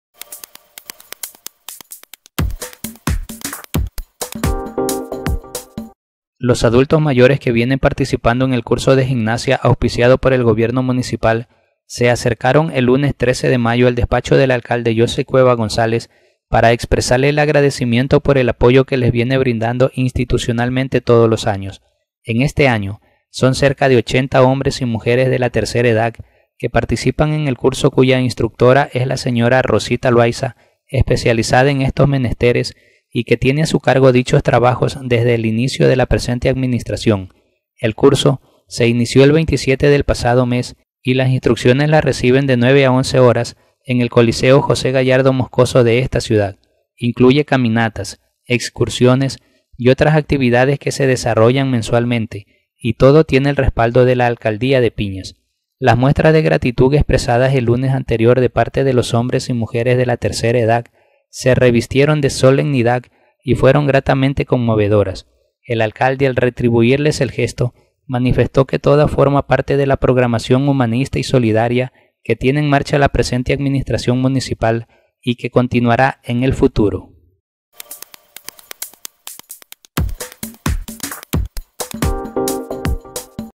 Adultos Mayores expresaron gratitud al Alcalde por el apoyo al curso de Gimnasia
Los adultos mayores que vienen participando en el curso de gimnasia auspiciado por el Gobierno Municipal, se acercaron el lunes anterior (13) al despacho del Alcalde Joseph Cueva González para expresarle el agradecimiento por el apoyo que les viene brindando institucionalmente todos los años